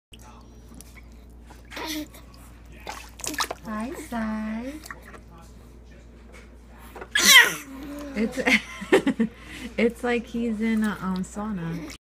Taking a bath
I love bath time! Mommy puts a cloth on my chest so I stay warm the whole time. It gets cold in there sometimes. Tonight she put a cloth on my head too. It made her laugh when I started talking about it.